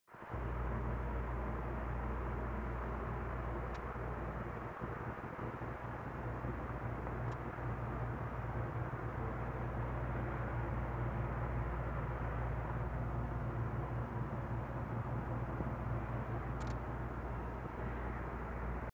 Curbside Furniture Flip
Craft supply haul, come see what I got from @AMakersStudio! Visit their website to find what I bought and more.